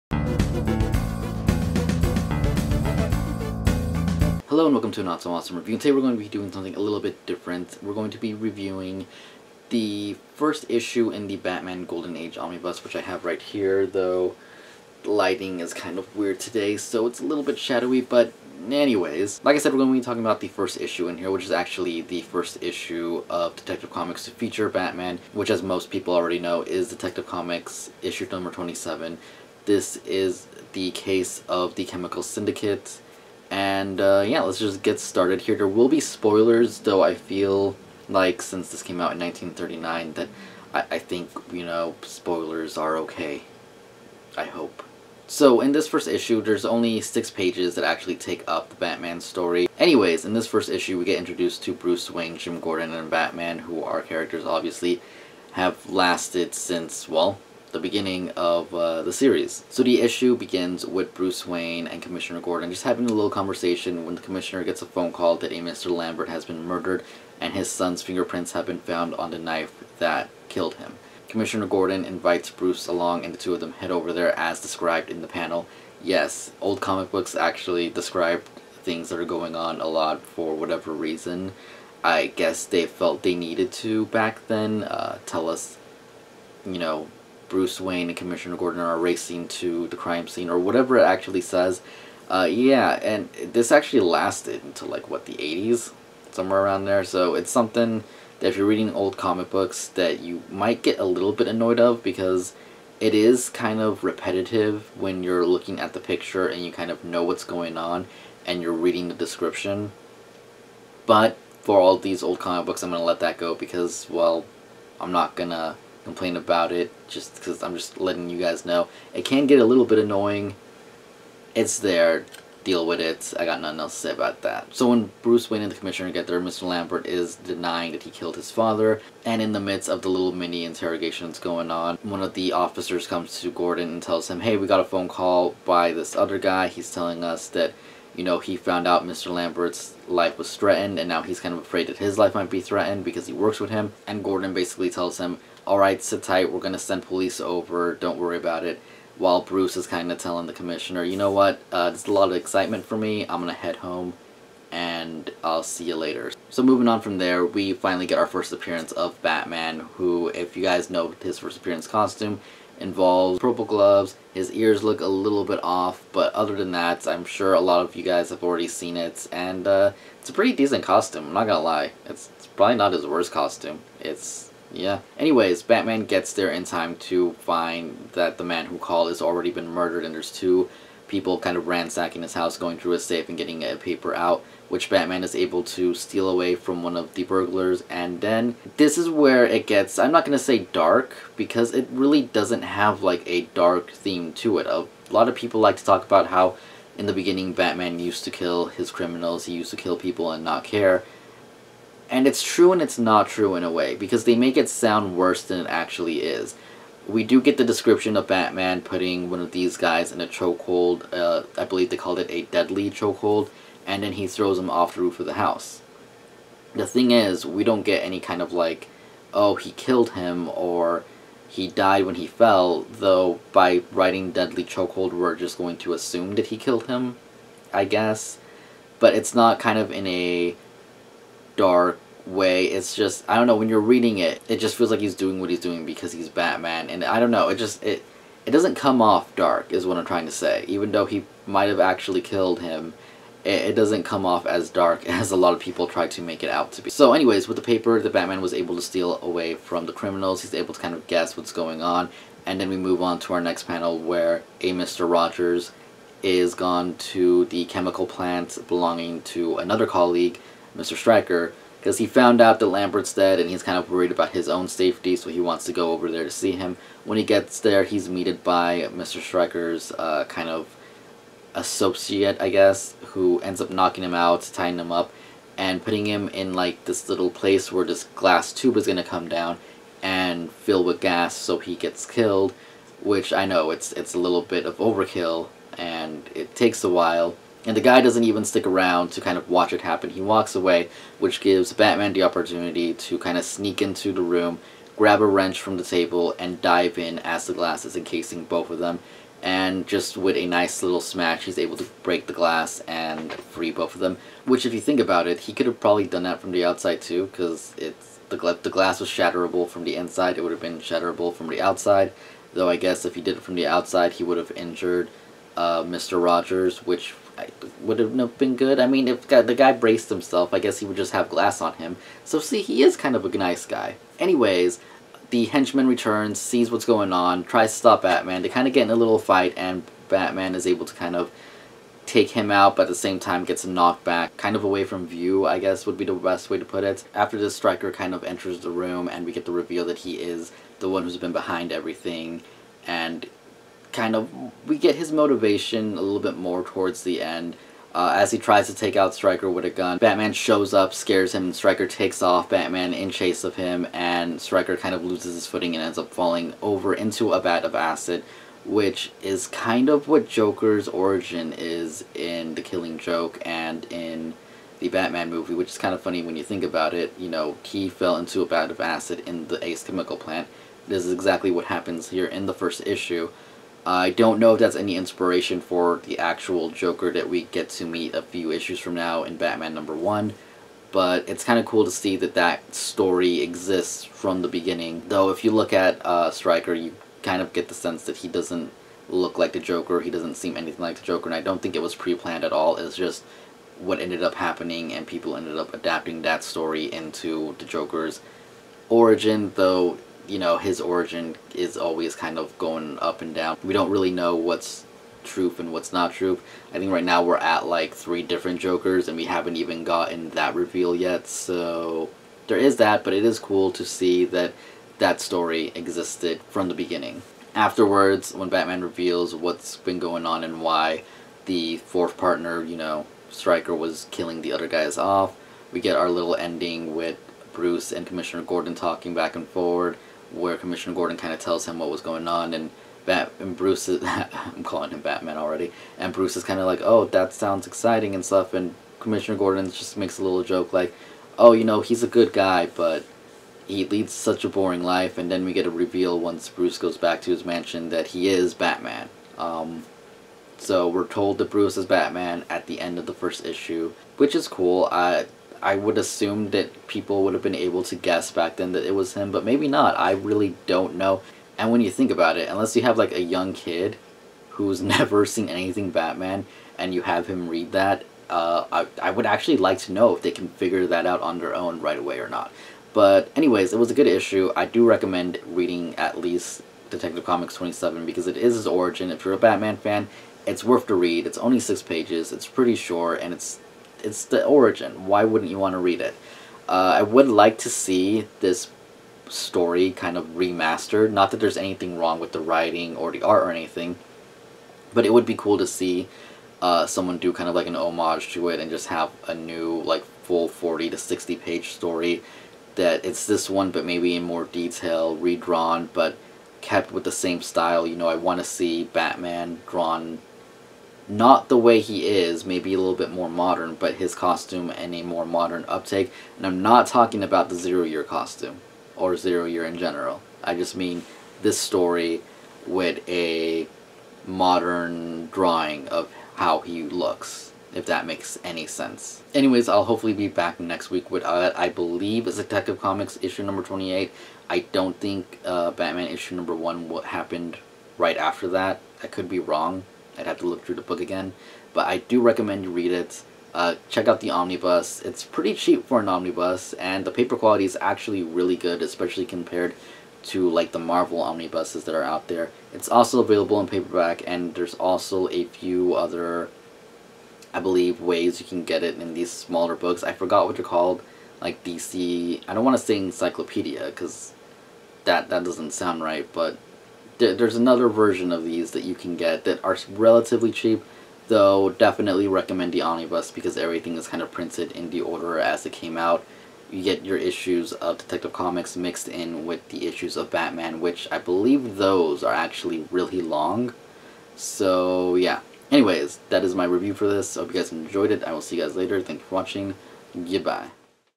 Detective Comics 27 | A Not So Awesome Review | Golden Age Review
A quick look at Detective Comics 27 from the pages of the Batman golden age omnibus vol 1